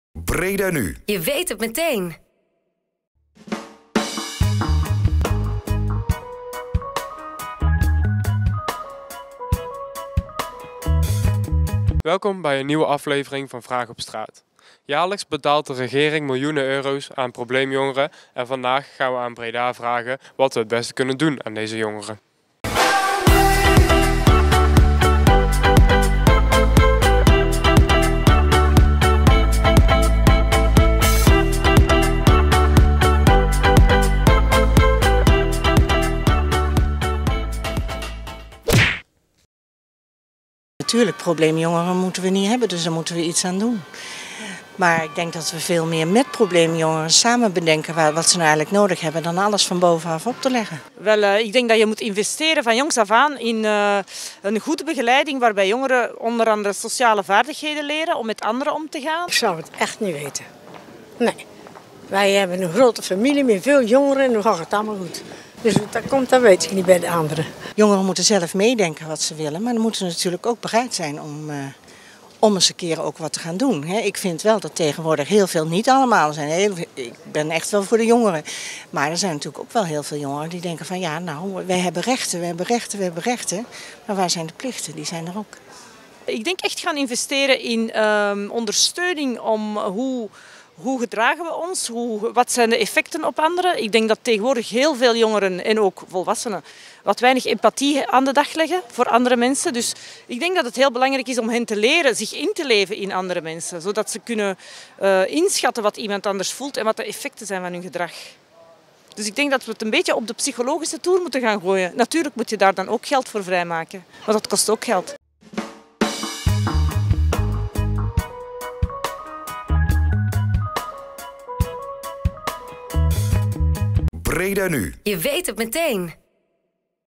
Vraag op Straat: Jeugdbendes
Nederland geeft iedere jaar miljoenen euro's uit aan het helpen van probleemjongeren, wij vroegen aan de mensen van Breda hoe dat beter kan.